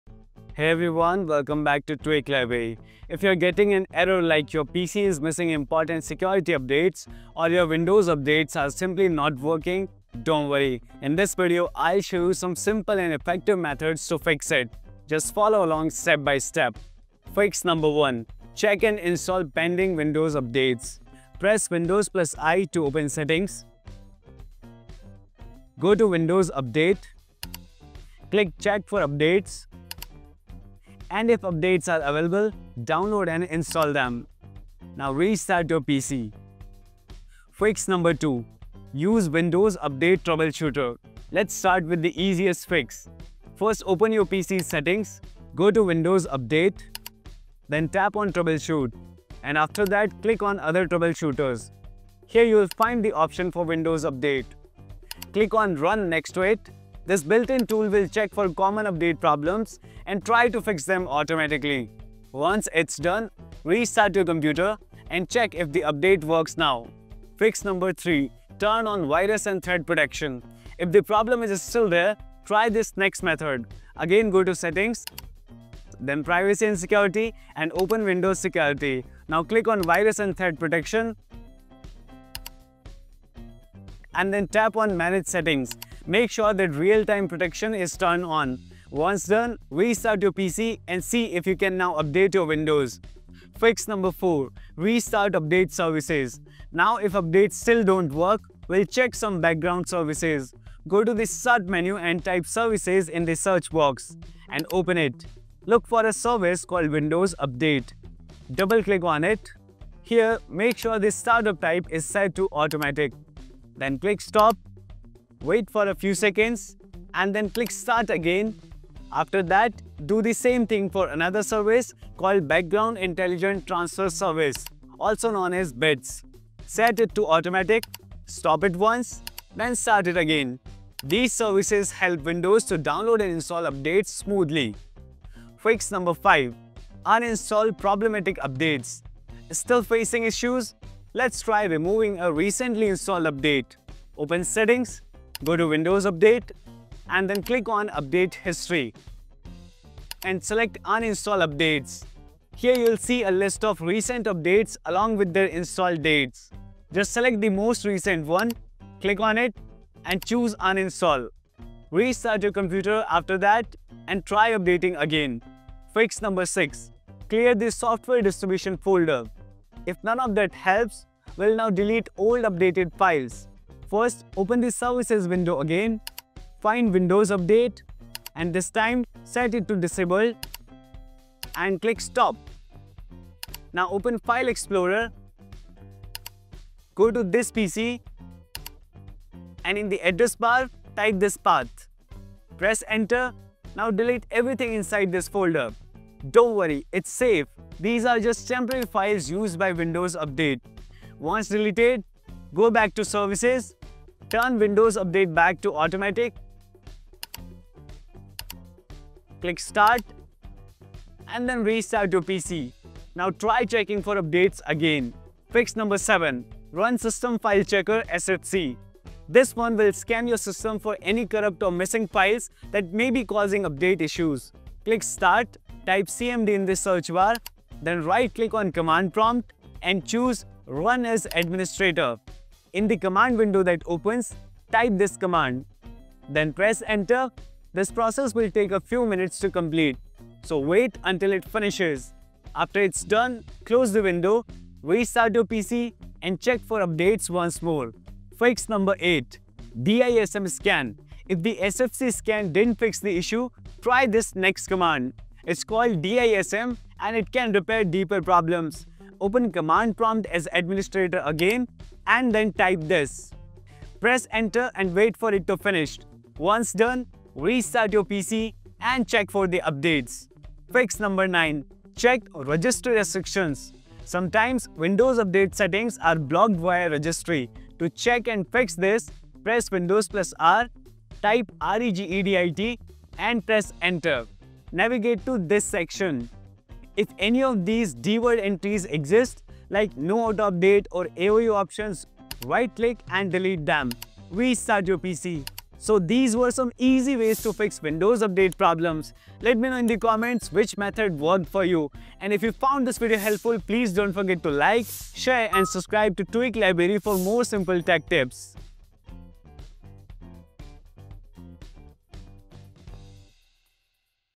Fix “Your Device is Missing Important Security Updates” Error in Windows 11/10
Are you seeing the error “Your device is missing important security updates” on your Windows 11 or Windows 10 PC? Don’t worry—this video will show you simple and effective ways to fix it. Whether it's due to paused updates, corrupted system files, or a network issue, we’ll walk you through step-by-step solutions to get your system up to date and secure. Watch now and resolve the issue in minutes!